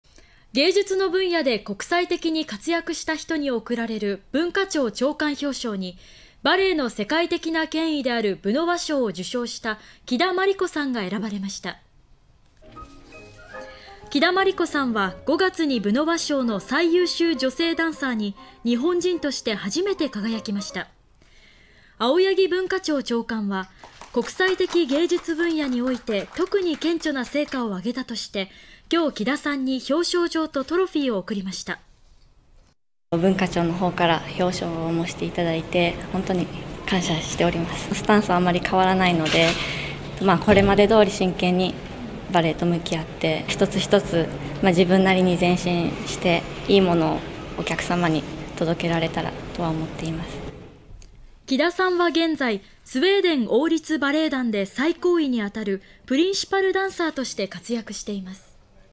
バレエ「ブノワ賞」の木田さんに文化庁長官表彰(14/08/07)
芸術の分野で国際的に活躍した人に贈られる文化庁長官表彰に、バレエの世界的な権威であるブノワ賞を受賞した木田真理子さん（31）が選ばれました。　木田さんは、5月にブノワ賞の最優秀女性ダンサーに日本人として初めて輝きました。